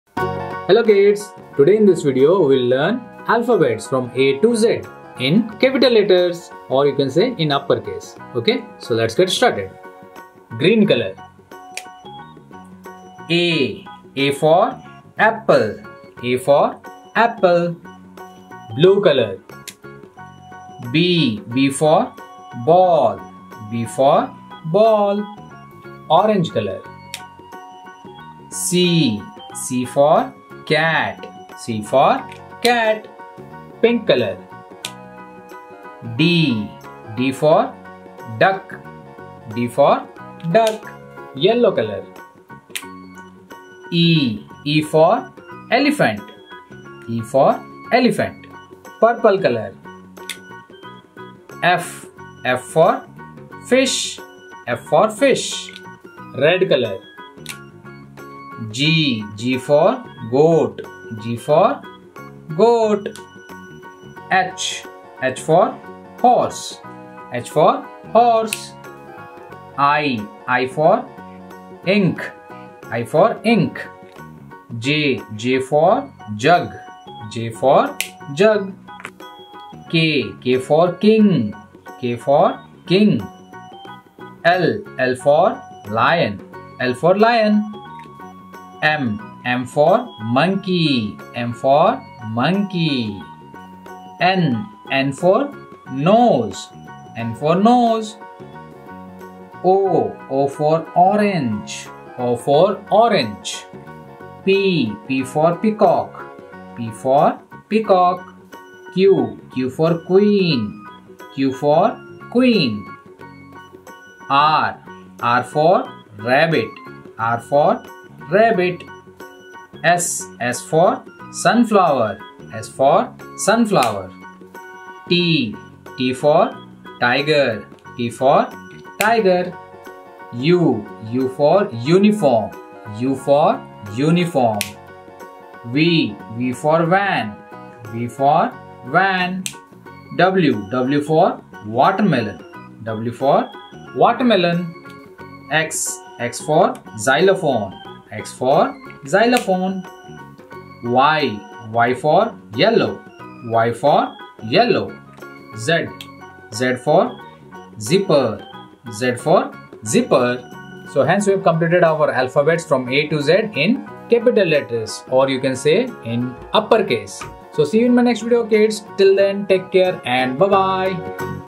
Learn to read and write Abcd by tracing the Alphabets for kids | A for Apple, B for Ball| Nursery
Learn to read and write Abcd by tracing the Alphabets for kids | A for Apple, B for Ball| Nursery classes
Hello Kids, Learn to write English Alphabets from A to Z by tracing Abcd with colorful Brush pens.